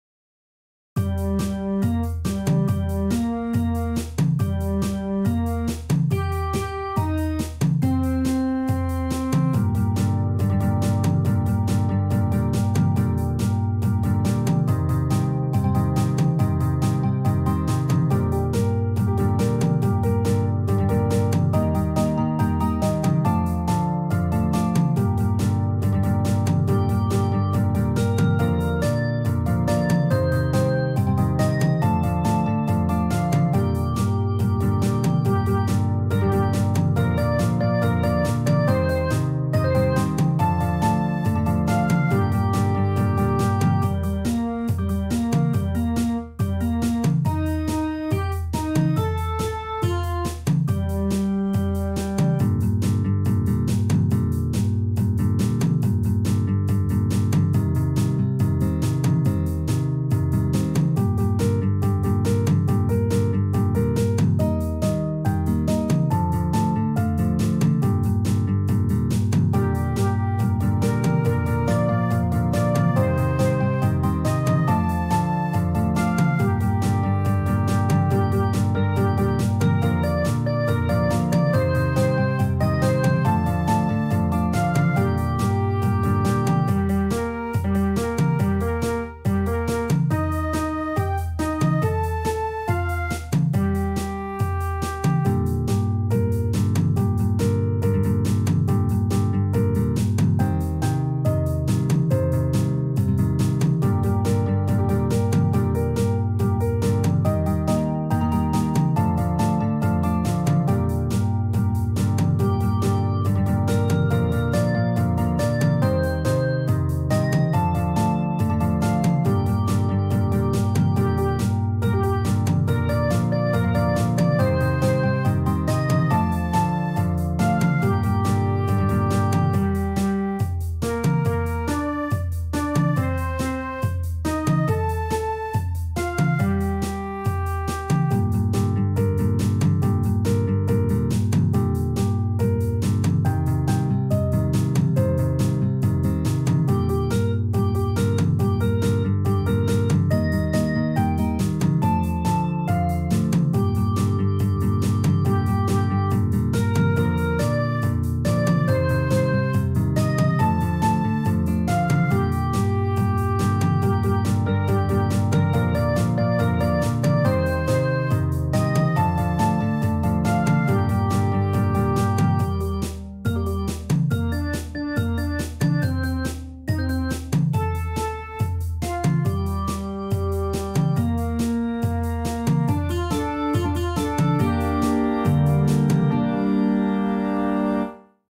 Daily Song February 24 with Sheet Music
Original song created today. Played in muse score with nylon guitar, drawbar organ, flute, piano, piano Rhodes(electric keyboard), and drums.

Created on Friday February 24, 2023.